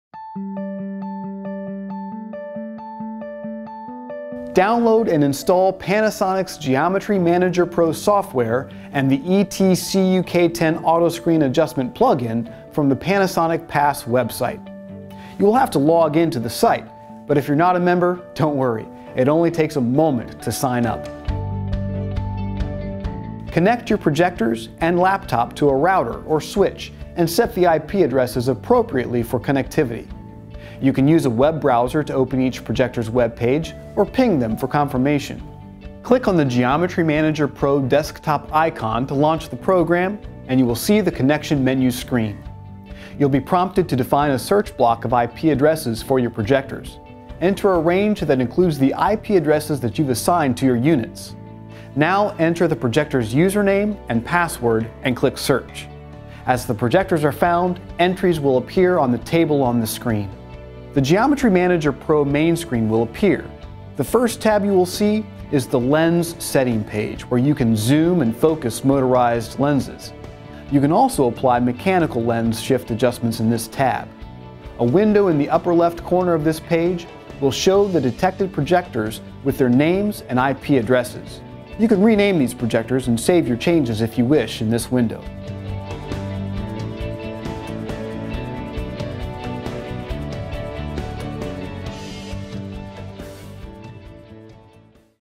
Panasonic ET-CUK10 Auto Screen Adjustment Software, Step 2: Networking Your Projectors
Learn how to use the Panasonic ET-CUK10 Auto Screen Adjustment Software. Step 2: Networking Your Projectors. Find out more on the ET-CUK10.